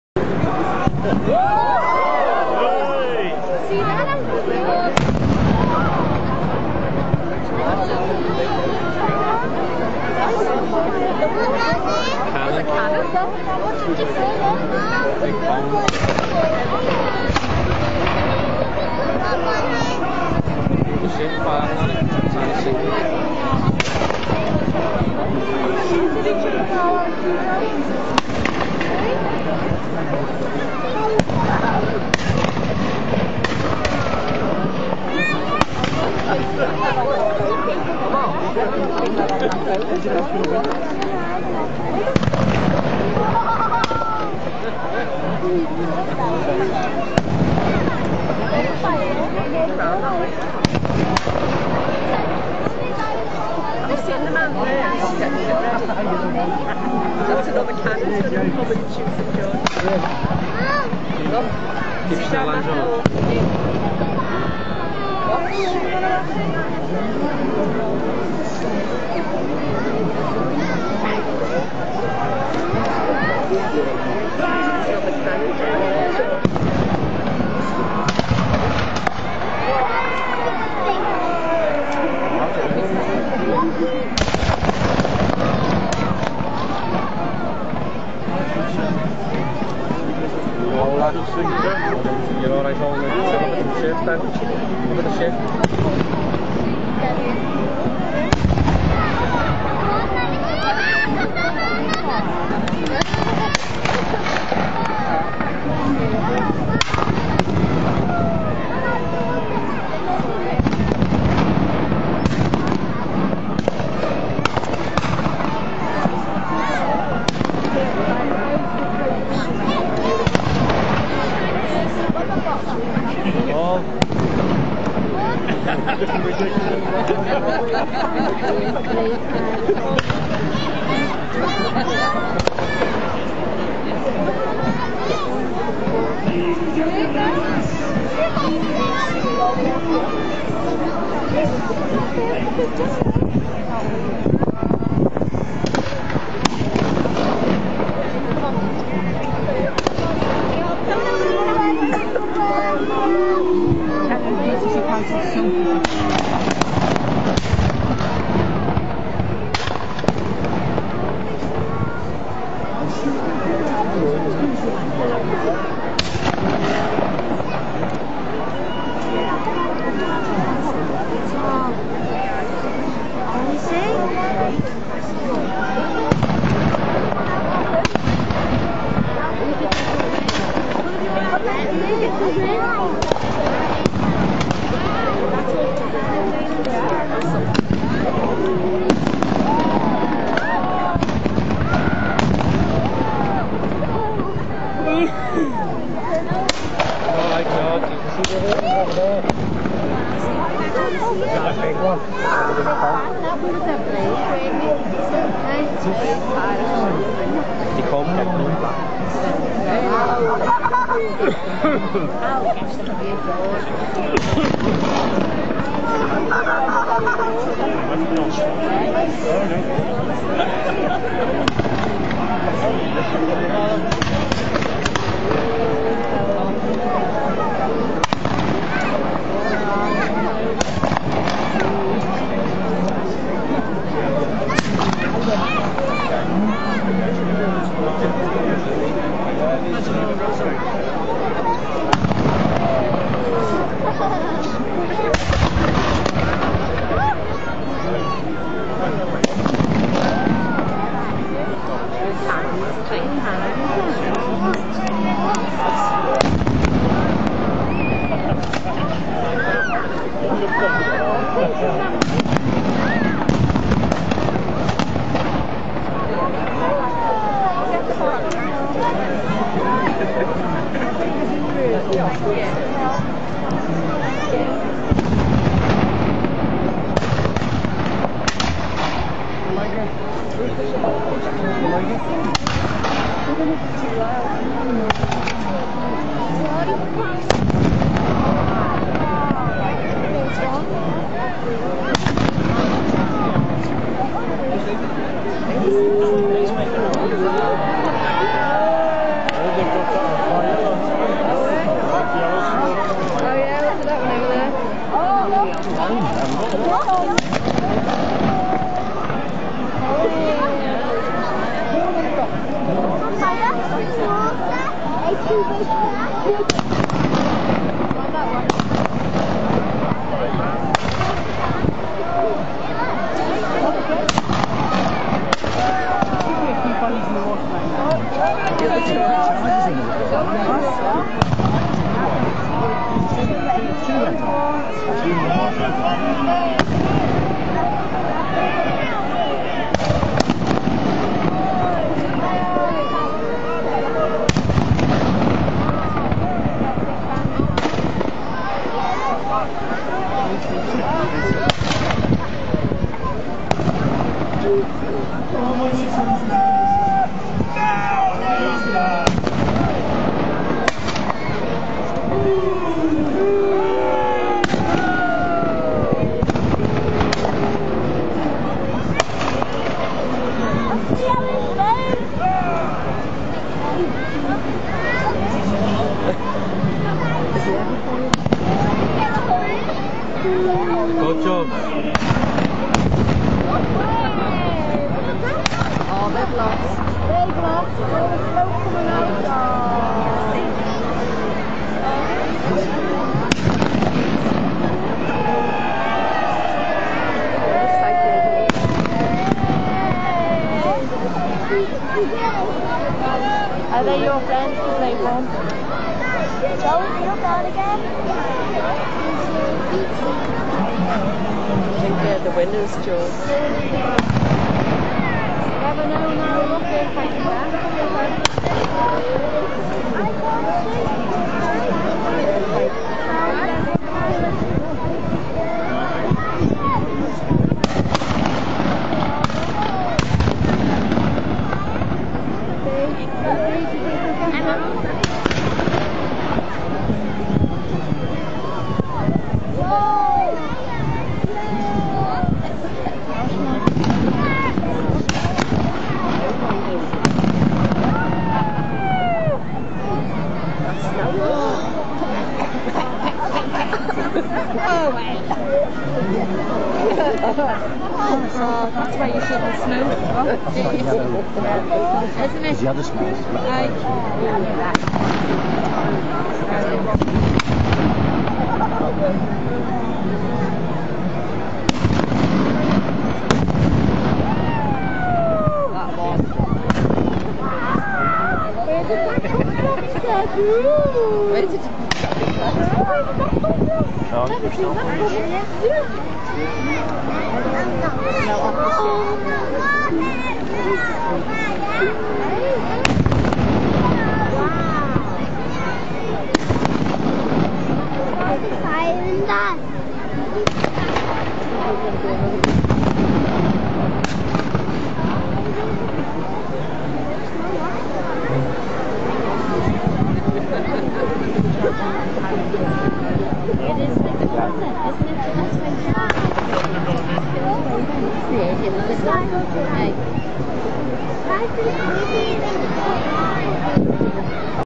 Pirate Battle 1 of 2.wmv
Pirate ships battle it out in Canning Dock at Liverpool's On The Waterfront festival 18th July 2010.  Cannons roar out, interspersed with musket fire.